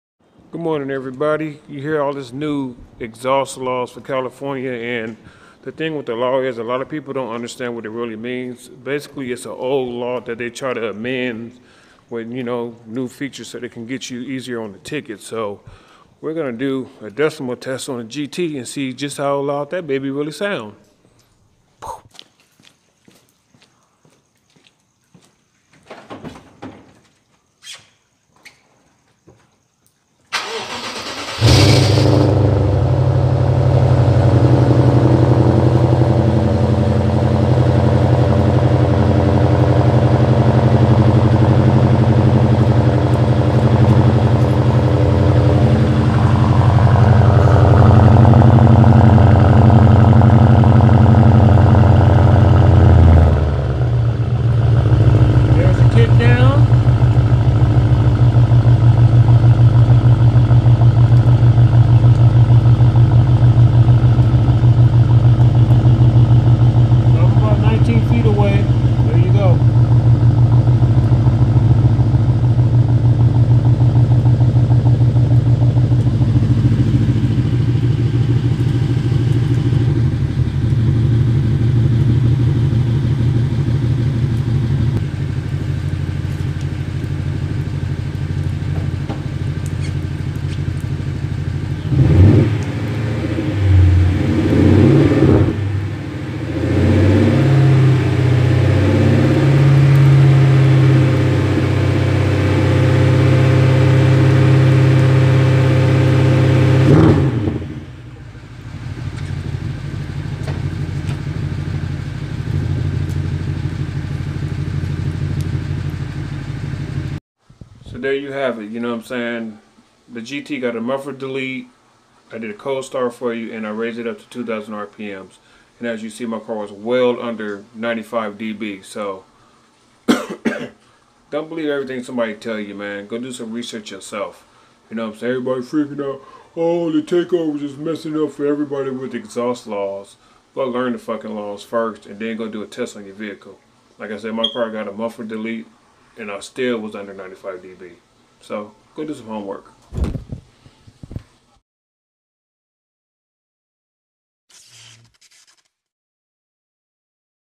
How loud is 95Db? On a Bmw twin turbo GT California's (AB)1824 Law and what it means for you
How loud is 95Db? California's (AB)1824 Law and what it means for you

Wanted to give some insight on what is 95Db's using an exhaust that is known to be loud. Hopefully you can use this a jump off point to see where you stand and what the law actually means. The is a lot of controversy on if the $1000 fine is real or what happens at State Ref. I thought I would actually take a few Db readings for examples.

CA Exhaust sound laws:
CA Emissions AB 1824